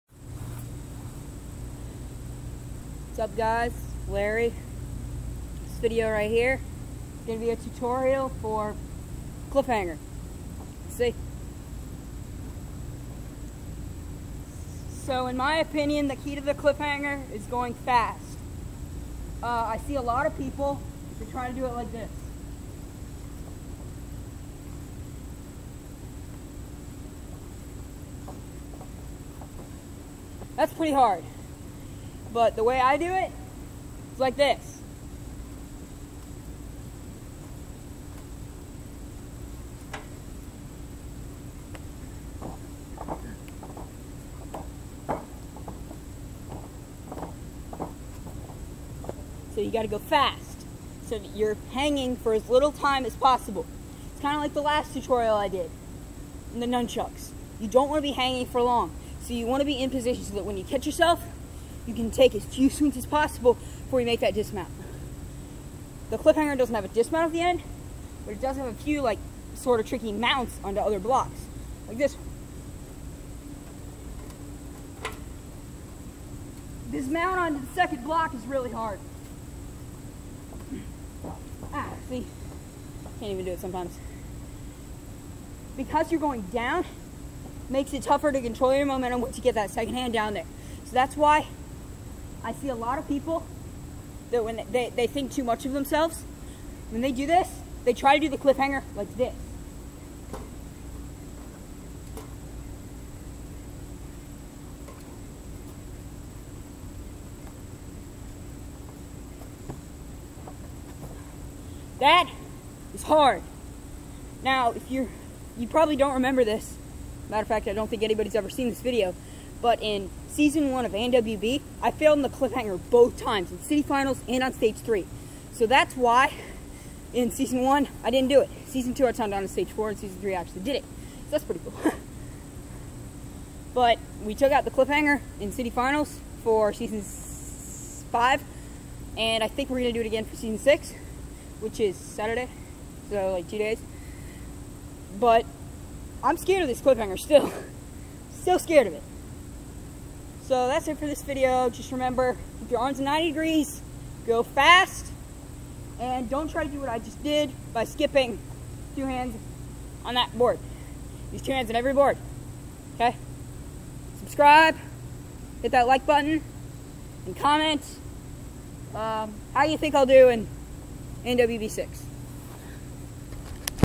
Cliffhanger tutorial (ANW)
This video will teach you how to do one of the toughest obstacles in ANW history, the cliffhanger.